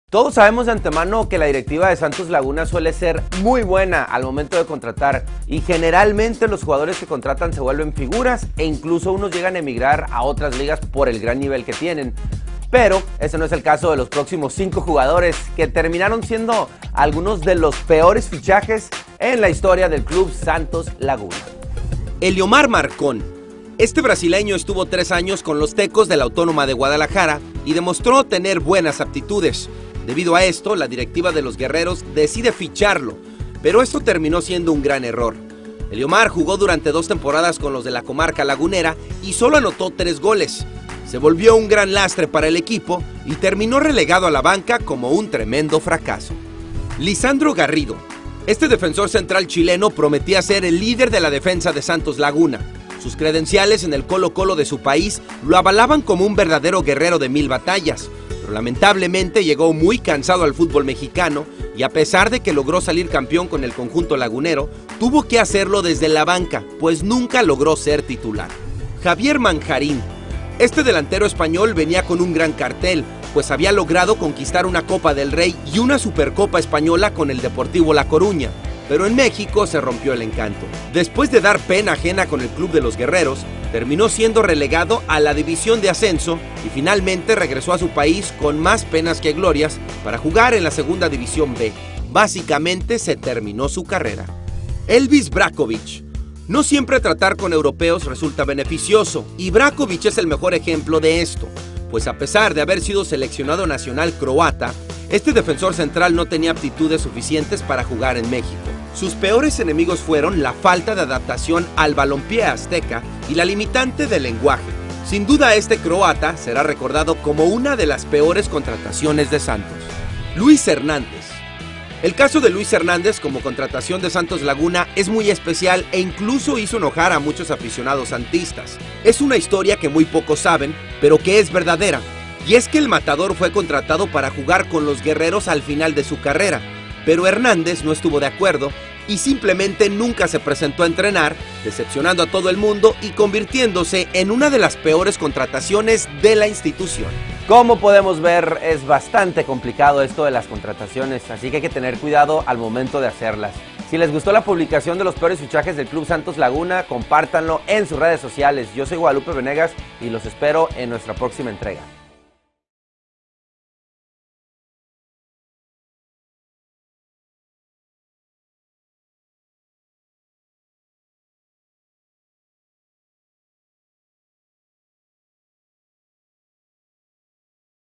Los peores fichajes de Santos Laguna | NBC Deportes
Video oficial de NBC Deportes. Santos Laguna suele ser muy bueno con sus contrataciones pero estos cinco jugadores terminaron siendo los peores fichajes en la historia del Club Santos Laguna.